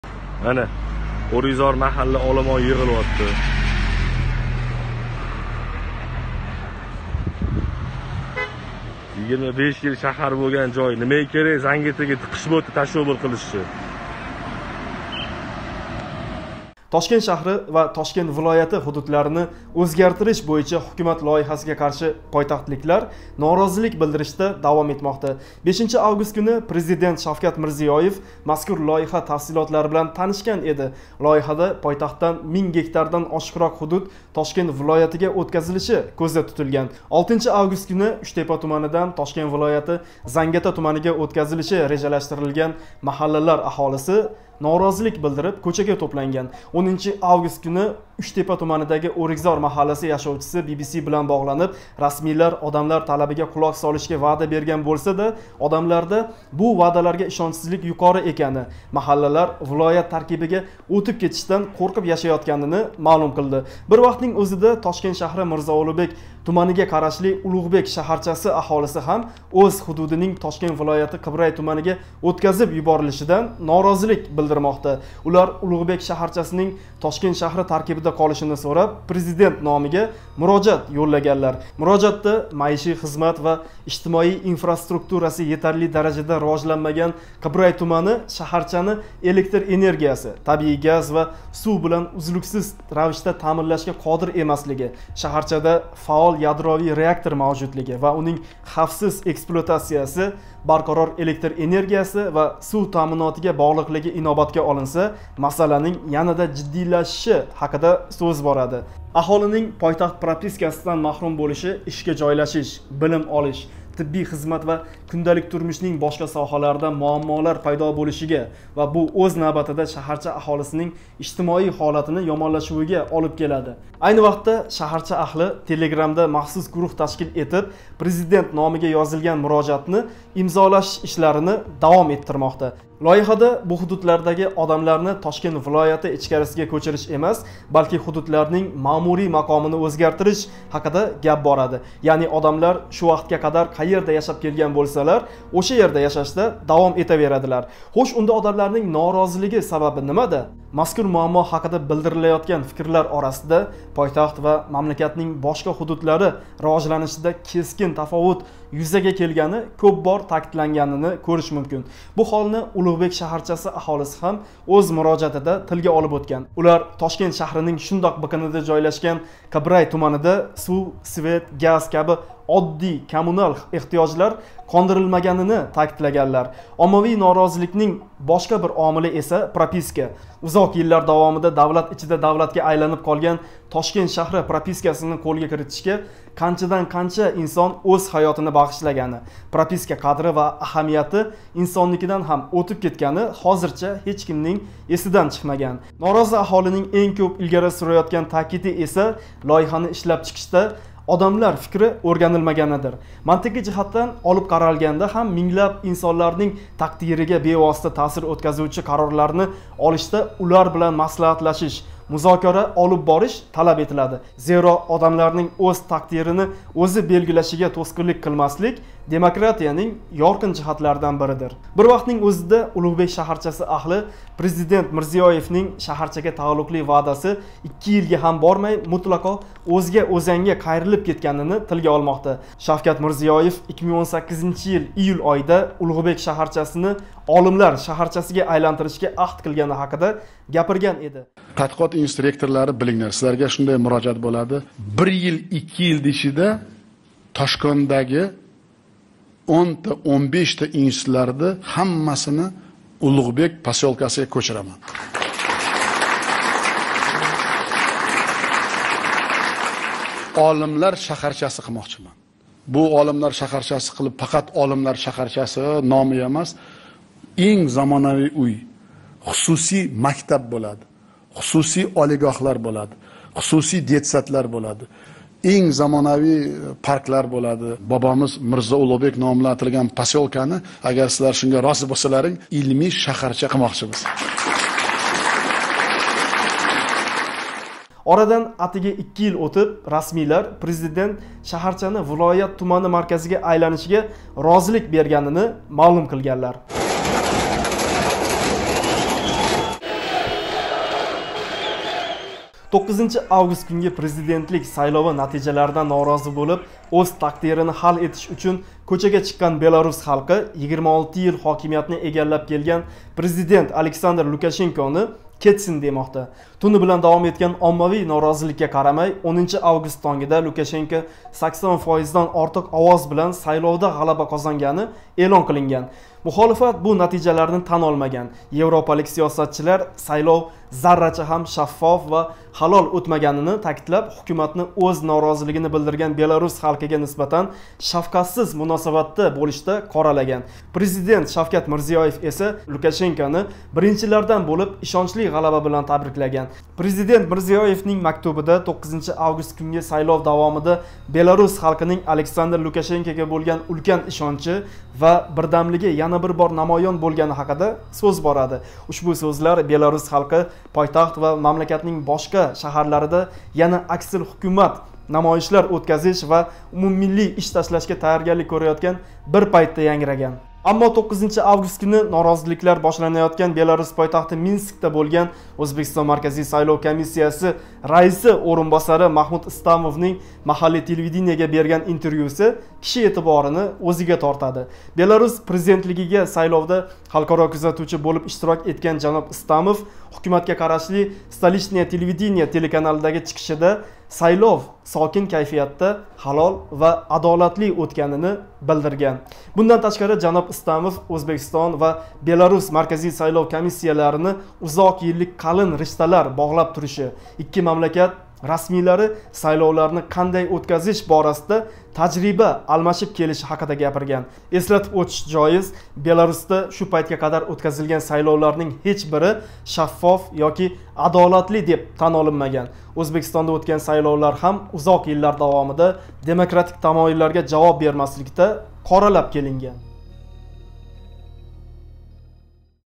​Тошкентликлар нимадан норози; Беларус ва Ўзбекистон - нима бирлаштириб туради? BBC News O'zbek​
Тошкент шаҳри ва Тошкент вилояти ҳудудларини ўзгартириш бўйича ҳукумат лойиҳасига қарши пойтахтликлар норозилик билдиришда давом этмоқда.

5 август куни президент Шавкат Мирзиёев мазкур лойиҳа тафсилотлари билан танишган эди.

Лойиҳада пойтахтдан минг гектардан ошиқроқ ҳудуд Тошкент вилоятига ўтказилиши кўзда тутилган.

6 август куни Учтепа туманидан Тошкент вилояти Зангиота туманига ўтказилиши режалаштирилган маҳаллалар аҳолиси норозилик билдириб, кўчада тўпланган.

10 август куни Учтепа туманидаги Ўрикзор маҳалласи яшовчиси Би-би-си билан боғланиб, расмийлар одамлар талабига қулоқ солишга ваъда берган бўлсада, аҳолида бу ваъдаларга ишончсизлик юқори экани, маҳаллалар вилоят таркибига ўтиб кетишдан қўрқиб яшаётганини маълум қилди.

Бир вақтнинг ўзида Тошкент шаҳри Мирзо Улуғбек туманига қарашли Улуғбек шаҳарчаси аҳолиси ҳам ўз ҳудудининг Тошкент вилояти Қибрай туманига ўтказиб юборилишидан норозилик билдирмоқда.

Президент Шавкат Мирзиёев Александр Лукашенконинг 9 август куни ўтган президентлик сайловидаги ғалабаси билан табриклаган.

9 август кунги президентлик сайлови натижаларидан норози бўлиб, ўз тақдирини ҳал этиш учун кўчага чиққан Беларус халқи 26 йил ҳокимиятни эгаллаб келган президент Александр Лукашенкони кетсин, демоқда.

Мухолифат бу натижаларни тан олмаган, европалик сиёсатчилар сайлов заррача ҳам шаффоф ва ҳалол ўтмаганини таъкидлаб, ҳукуматни ўз норозилигини билдирган белорус халқига нисбатан шафқатсиз муносабатда бўлишда қоралаган.

Аммо 9 август куни Беларус президентлигига сайловда халқаро кузатувчи бўлиб иштирок этган Ўзбекистон Марказий сайлов комиссияси раиси ўринбосари Маҳмуд Истамов маҳаллий телевидениега берган интервьюсида сайлов «сокин кайфиятда, ҳалол ва шаффоф ўтаётгани»ни билдирган.

Бундан ташқари жаноб Истамов Ўзбекистон шу пайтга қадар ўтказилган сайловларининг ҳеч бири шаффоф ёки адолатли, деб тан олинмаган Беларус Марказий сайлов комиссияси билан сайловларни қандай ўтказиш борасида тажриба алмашиб келиши ҳақида гапирган.